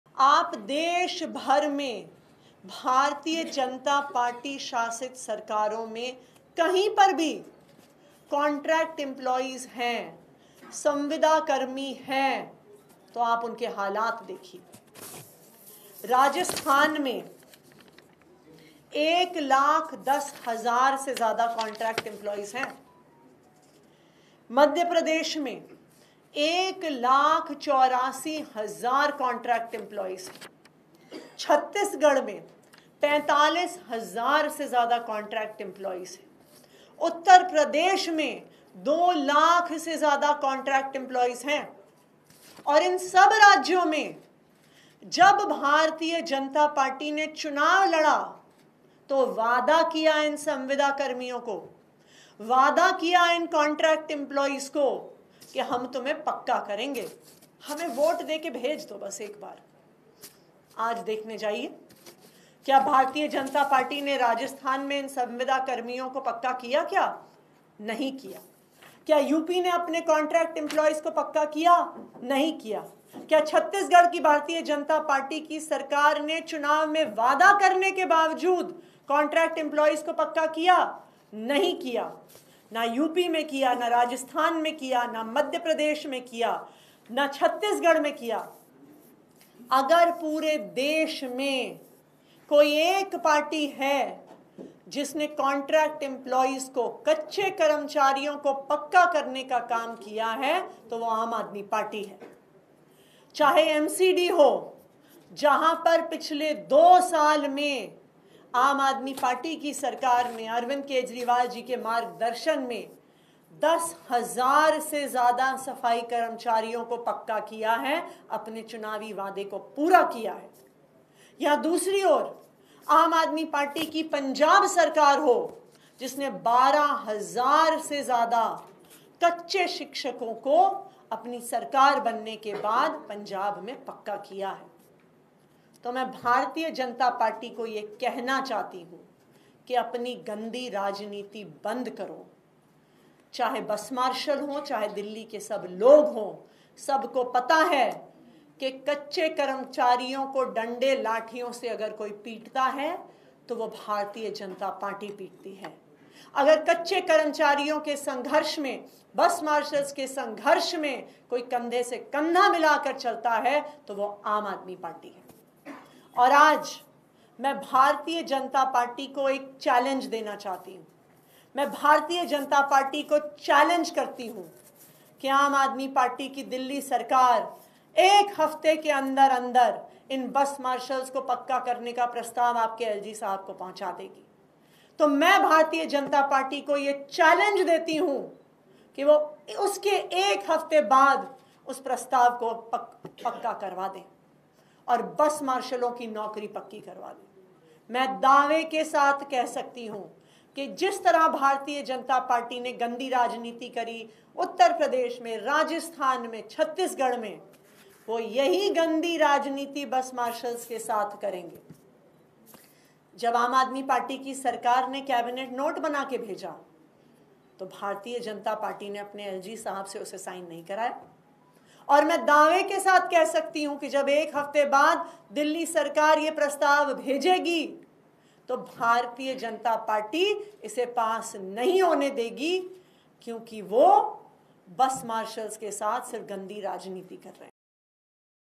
Delhi: दिल्ली CM Atishi का BJP को बड़ा चैलेंज, एक सप्ताह में भेजेंगे LG को Bus Marshal की फाइल
⏩दिल्ली CM आतिशी का BJP को बड़ा चैलेंज, एक सप्ताह में भेजेंगे LG को बस मार्शल की फाइल

⏩"पूरे देश में BJP शासित राज्यों में कॉन्ट्रैक्ट कर्मचारियों के हालात खराब"

⏩"चुनाव से पहले किया था पक्के करने का वादा, अब तक नही किया पूरा"

⏩AAP ने दो साल में MCD के 10 हजार से ज्यादा कॉन्ट्रैक्ट पर लगे सफाई कर्मचारियों को पक्का किया

⏩"पंजाब में भी AAP की सरकार ने 12 हजार से ज्यादा कॉन्ट्रैक्ट शिक्षकों को किया पक्का"

⏩बस मार्शल पर अपनी गंदी राजनीति बंद करे BJP